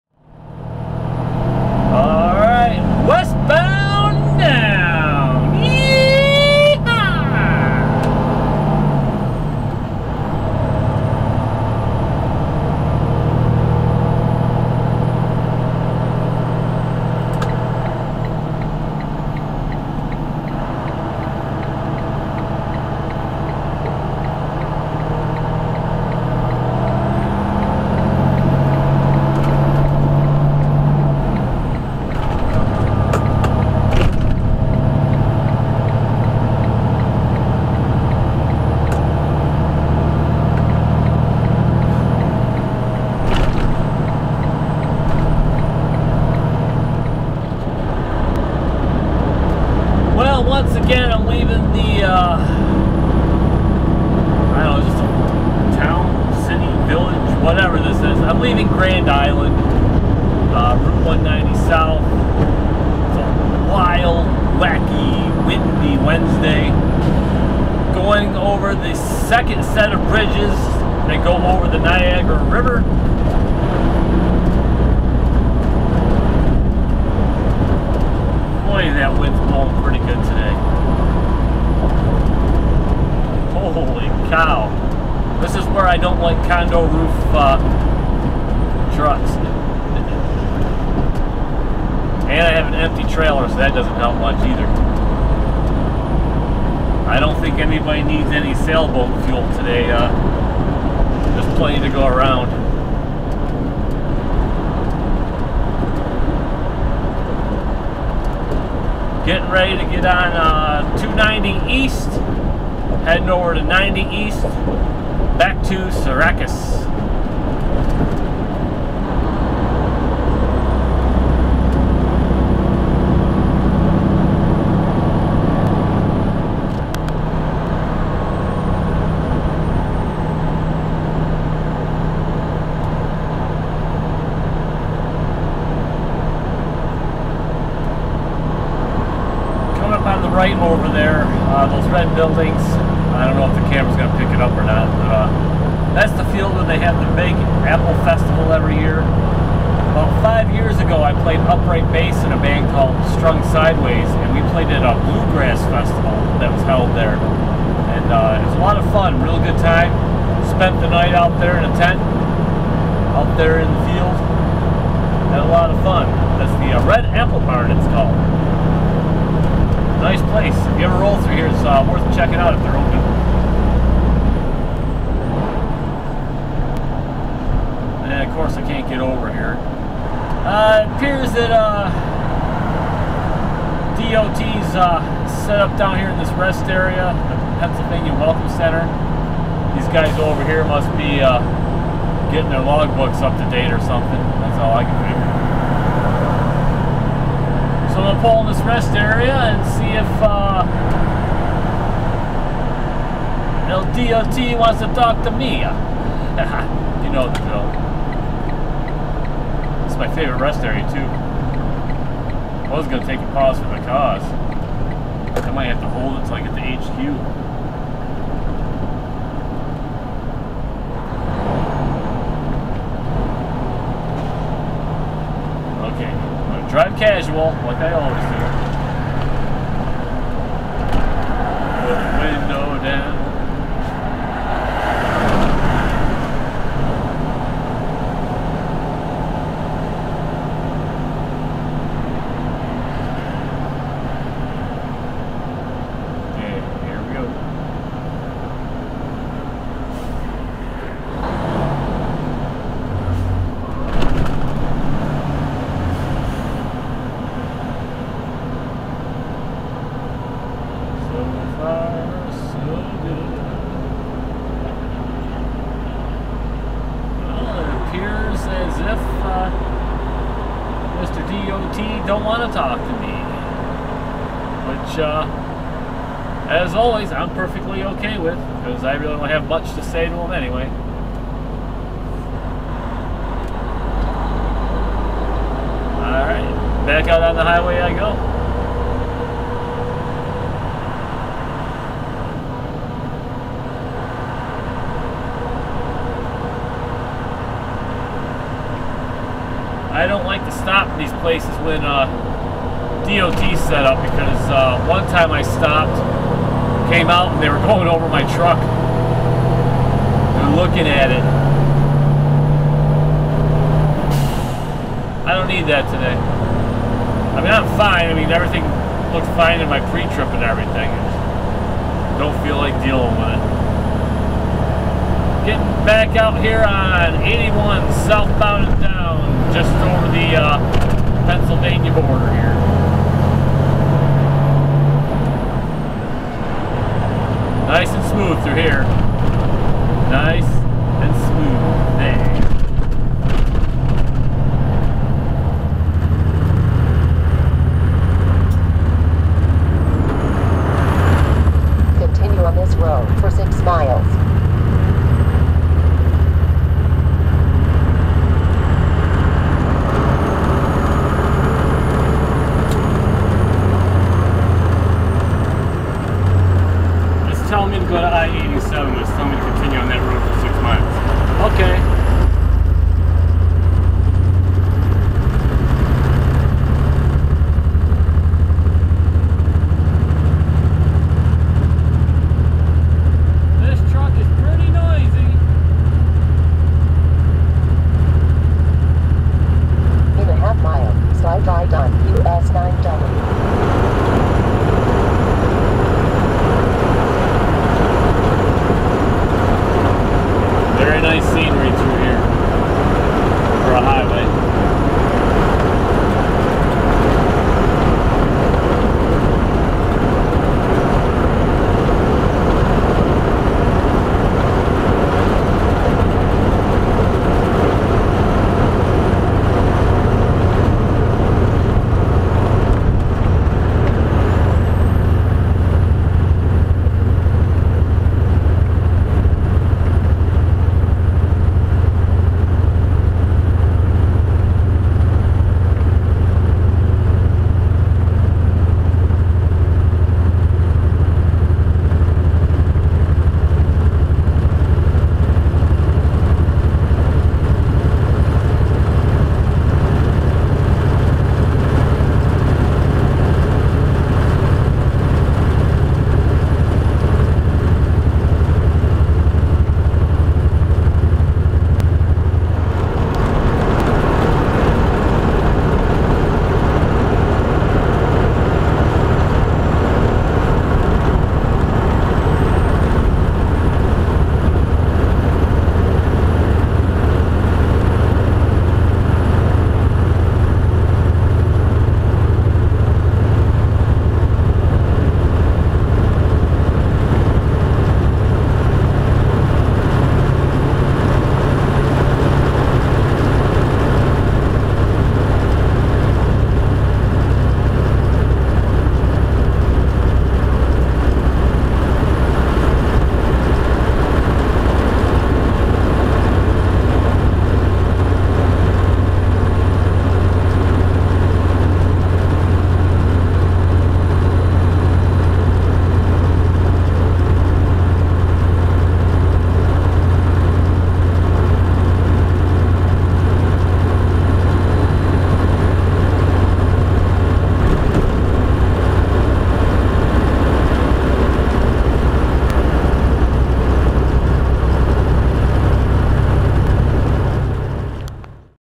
Left Overs 3:Clips that didn't make last week's vlogs
I'm a local/short haul truck driver...and these are the MisAdventures of my travels down the road that I share with y'all.